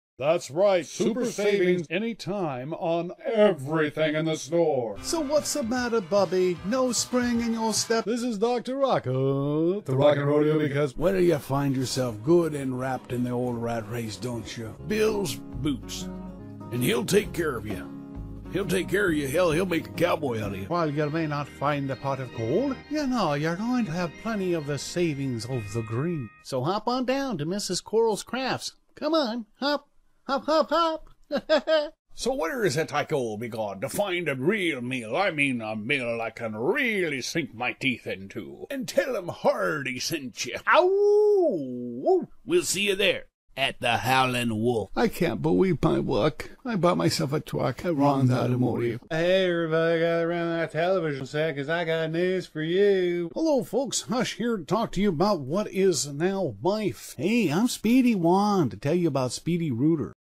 VO Vid Commercial Moment
Voice Over by Kevin Wolf of Portland Puppets.

The Portland Puppets create videos and perform live shows. And has the largest private voice over bank of voices for you to choose from! Over 100 Different Character Voices.